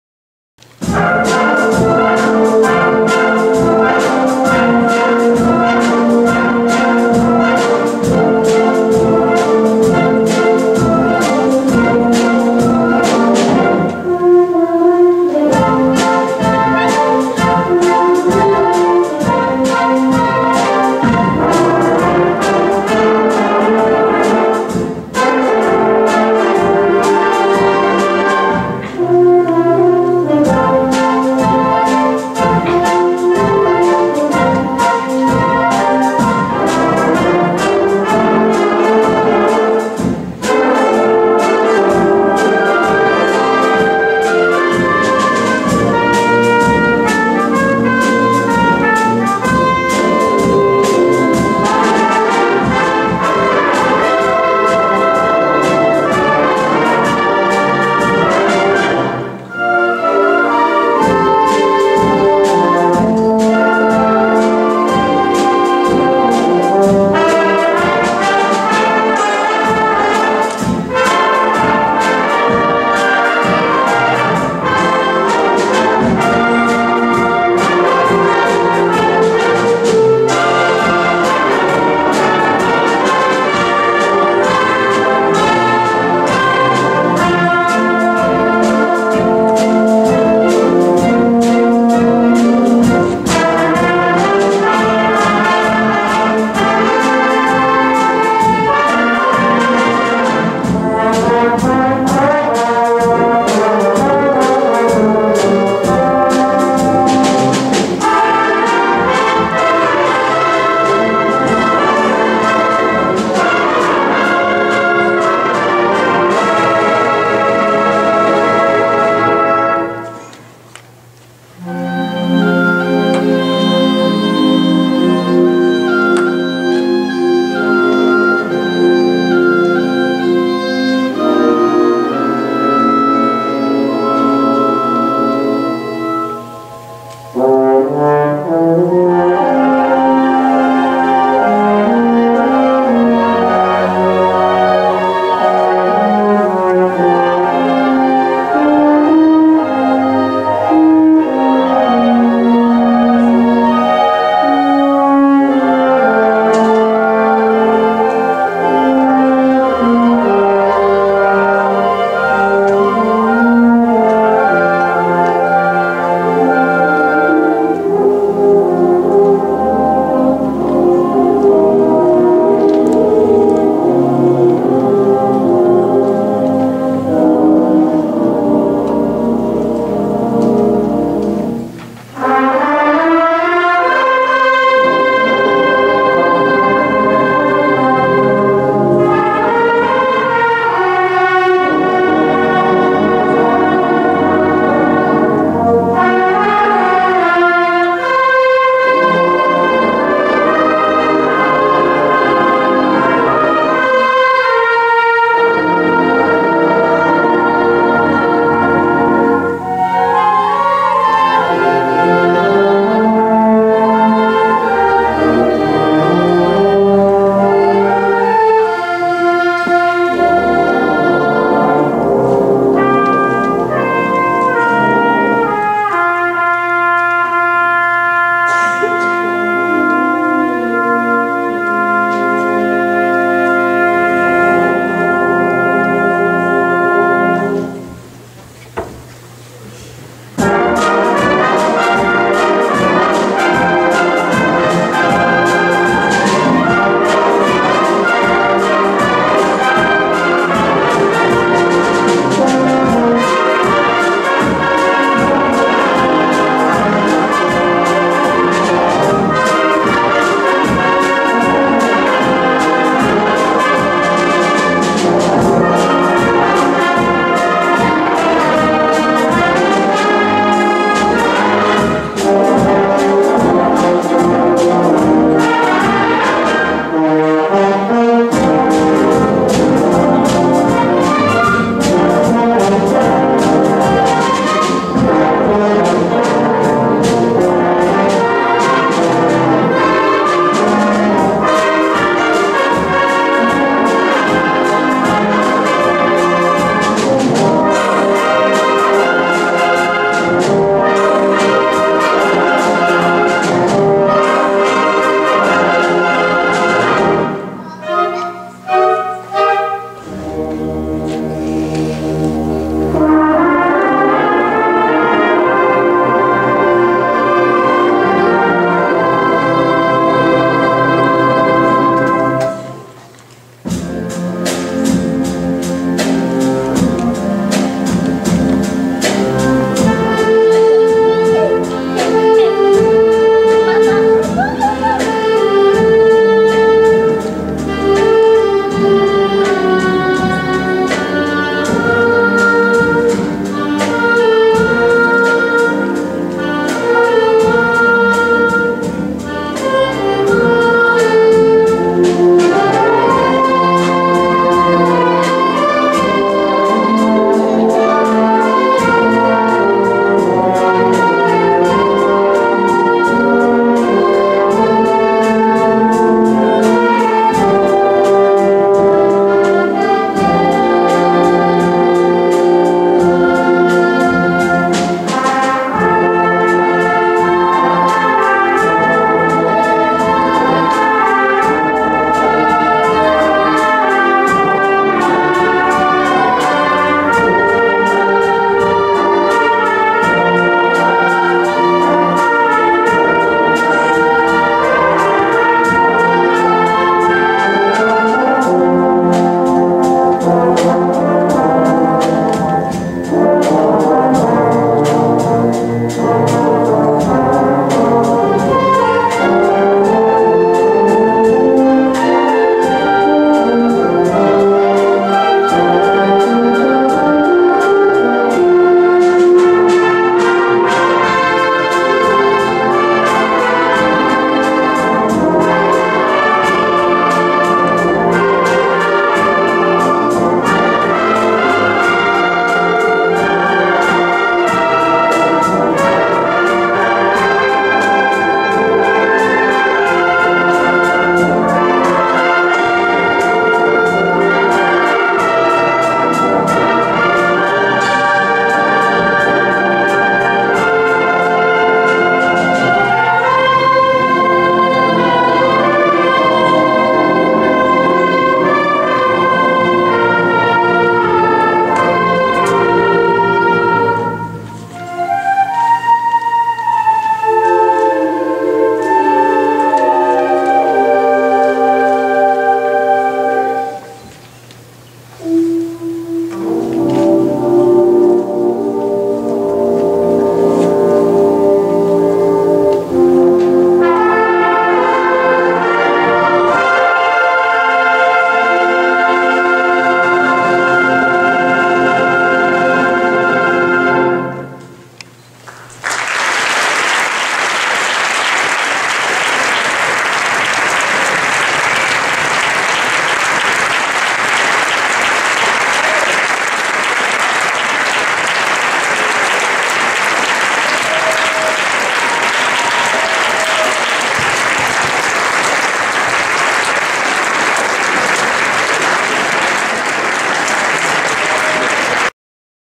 Frank Sinatra in Concert
Final Concert of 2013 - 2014 -Blue Symphonic Band